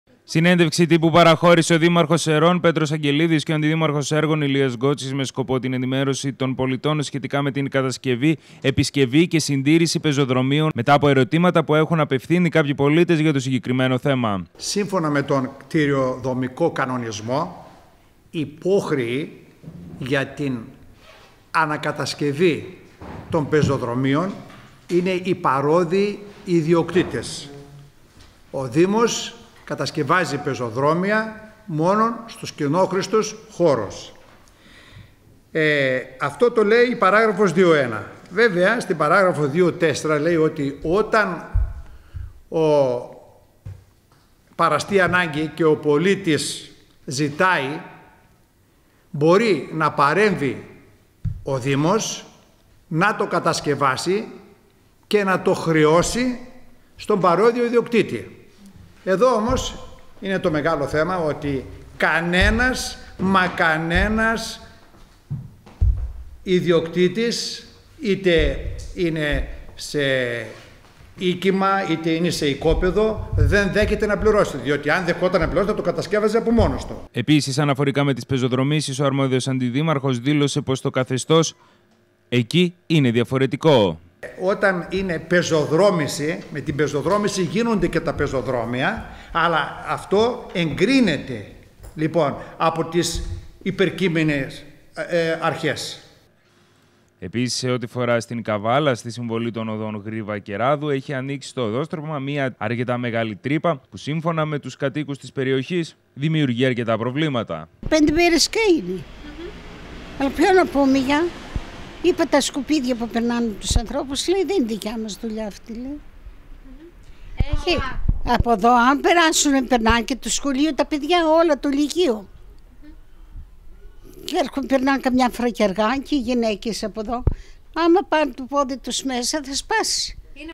Δηλώσεις του Αντιδημάρχου έργων και ανάπτυξης για τα κατεστραμμένα πεζοδρόμια στις Σέρρες
Συνέντευξη τύπου παραχώρησε ο δήμαρχος Σερρών Πέτρος Αγγελίδης και ο αντιδήμαρχος έργων Ηλίας Γκότσης με σκοπό την ενημέρωση των πολιτών σχετικά με την κατασκευή επισκευή και συντήρηση πεζοδρομίων μετά από ερωτήματα που έχουν απευθύνει κάποιοι πολίτες για το συγκεκριμένο θέμα. Αναφορικά με τις πεζοδρομήσεις ο αρμόδιος αντιδήμαρχος δήλωσε πως το καθεστώς εκεί είναι διαφορετικό.